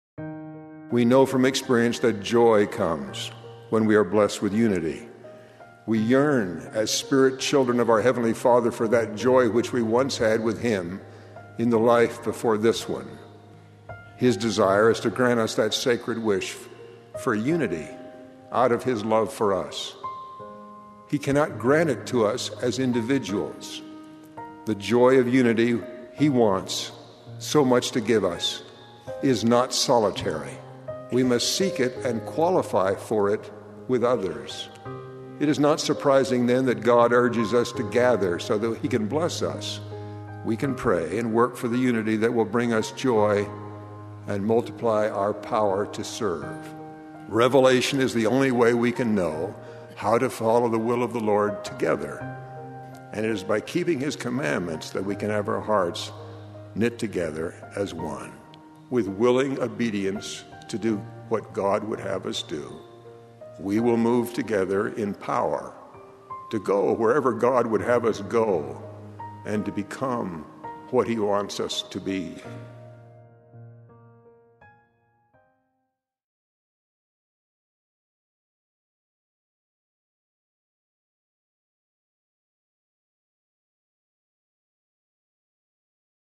Uniti in rettitudine
Quando obbediamo ai comandamenti di Dio e abbiamo i nostri cuori legati in unità, possiamo essere uniti in rettitudine.